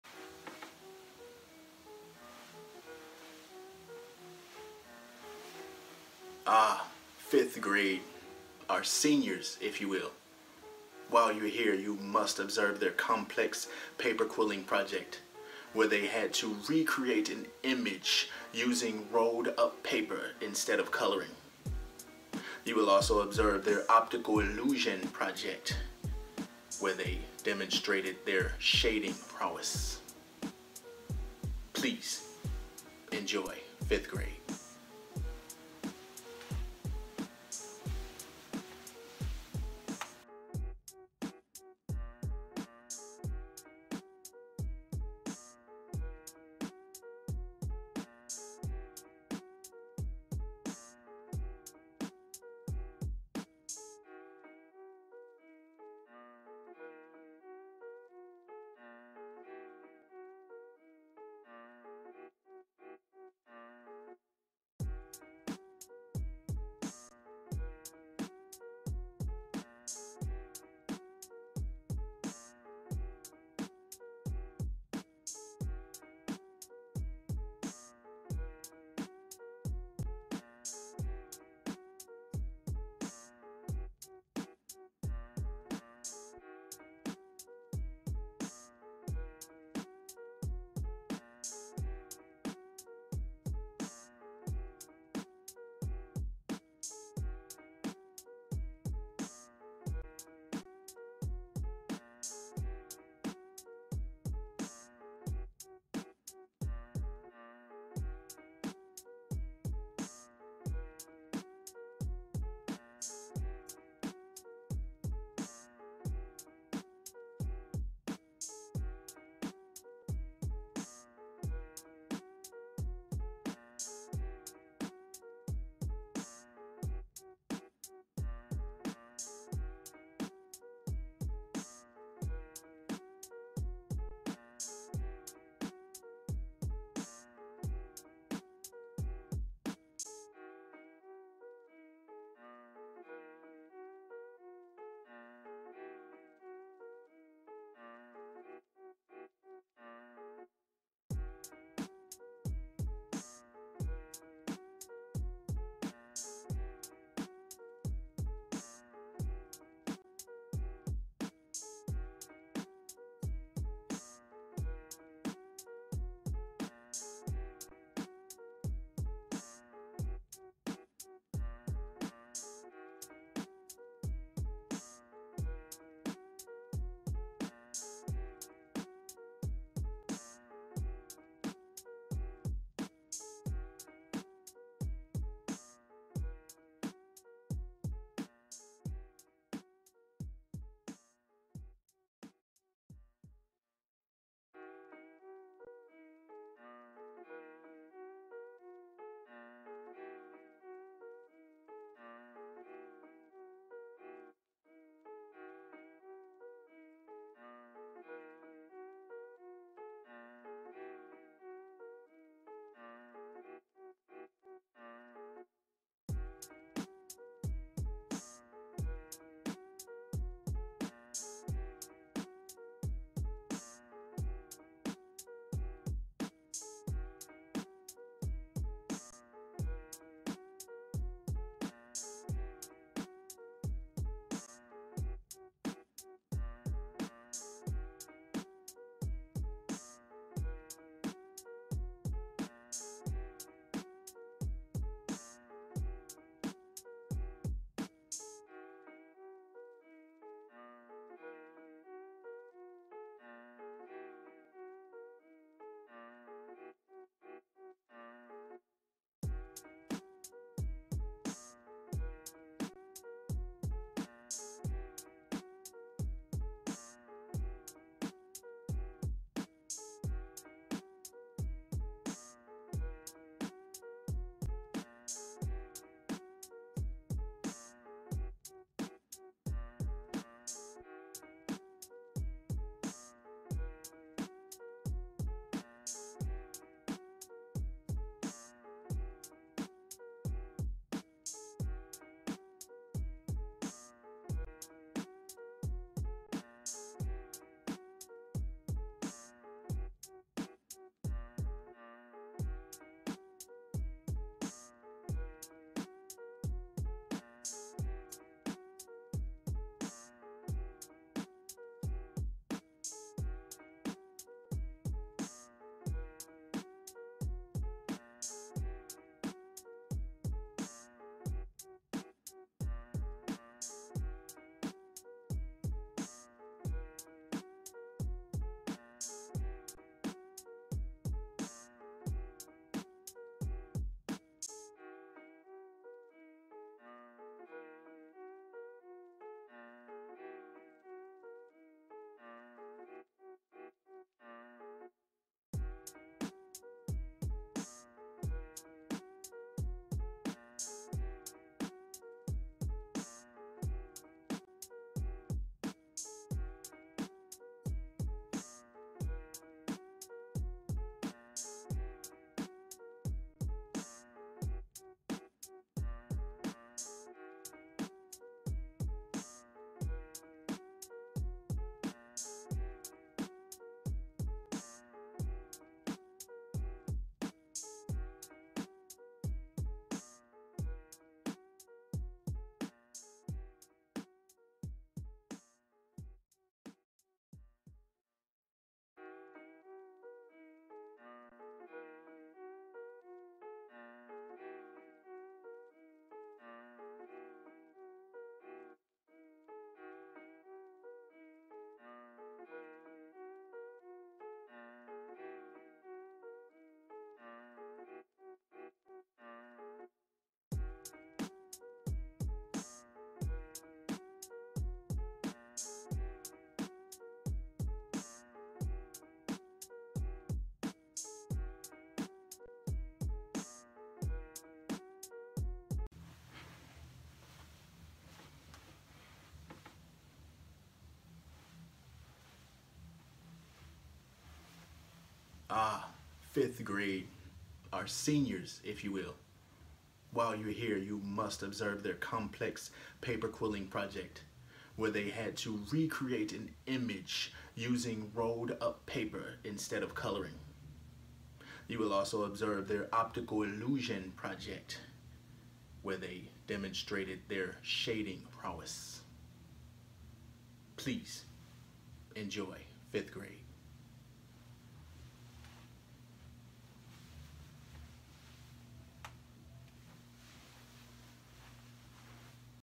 5th Grade Art Fair 2020
1st official, unofficial Art Fair presentation for the students, parents and guardians.

I had all that artwork collected, so I wanted to do something with it for the students, before I gave it back to the homeroom teachers to give back.

This is everything I had graded and cataloged before our release